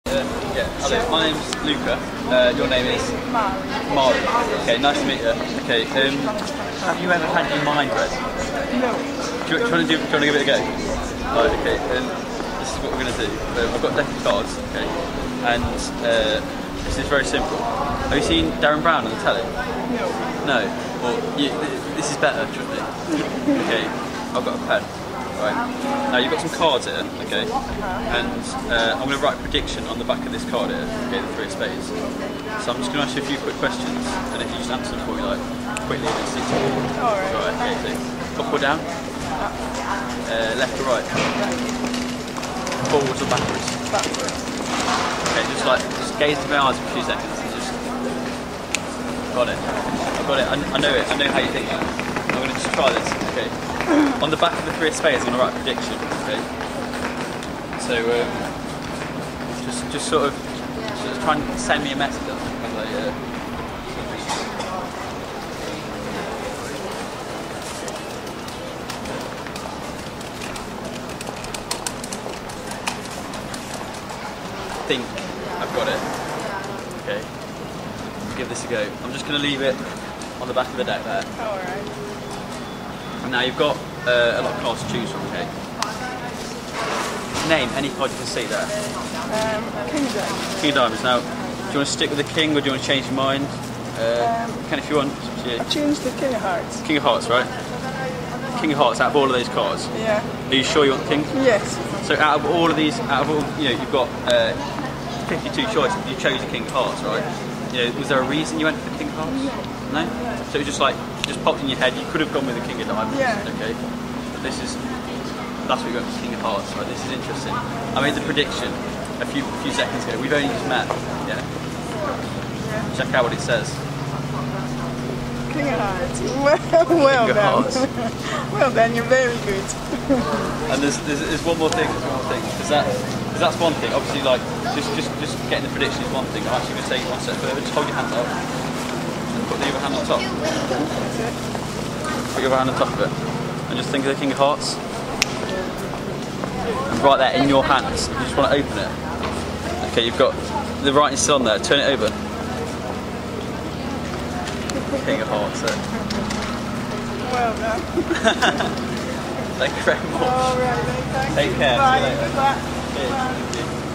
Street magic- Video 1- magician Luca Gallone wows the public
Watch as young magician Luca Gallone takes to the streets of Derby for his first instalment of street magic.